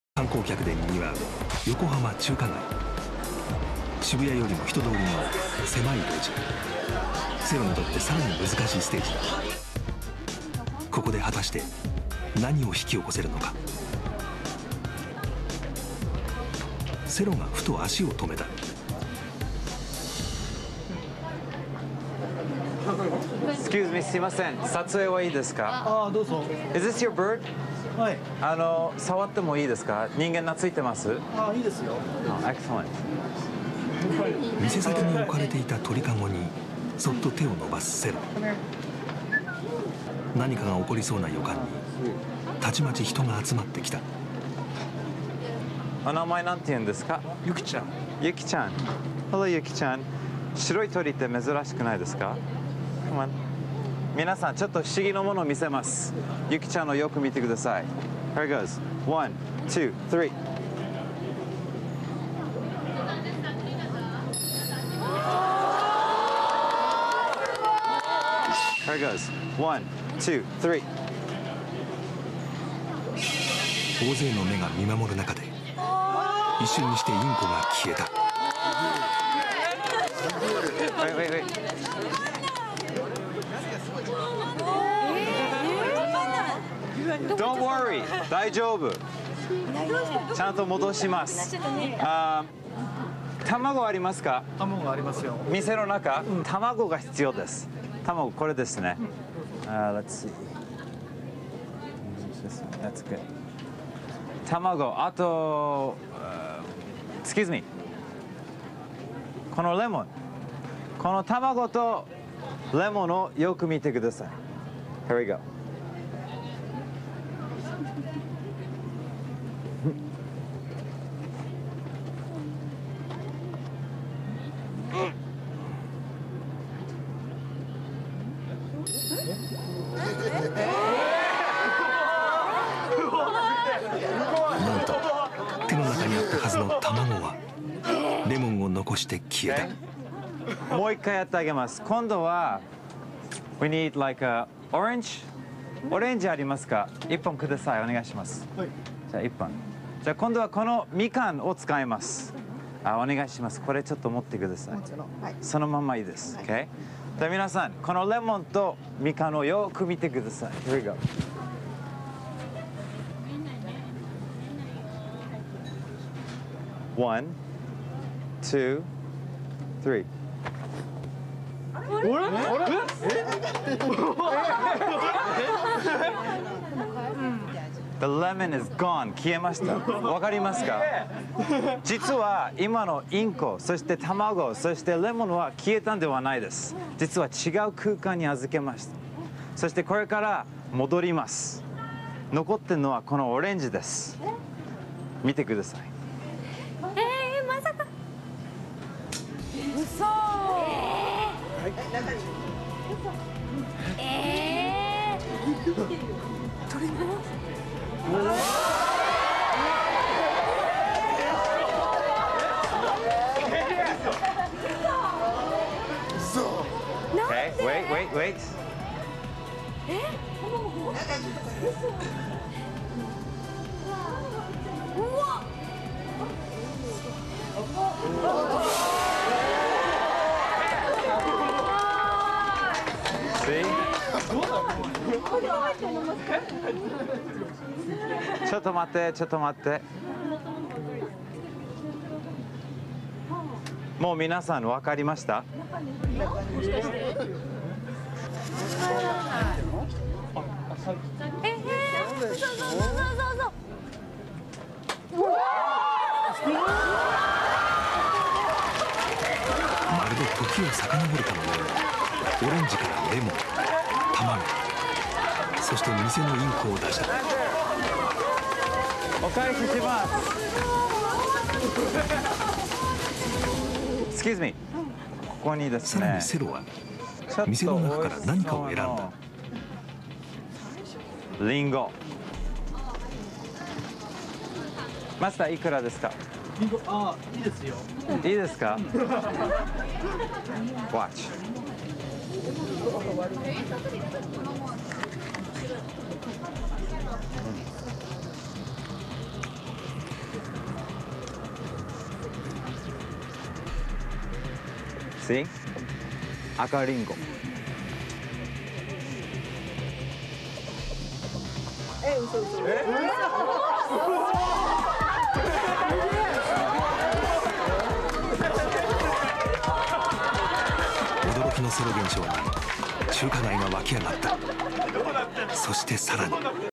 Cyril Takayama - China Town Dove Magic
Cyril Takayama (born September 27, 1973) is an American illusionist of Japanese and French descent. He is a member of the short-lived television series T.H.E.M.(2004).

Sorry..about the missing subtitle and rather bad quality.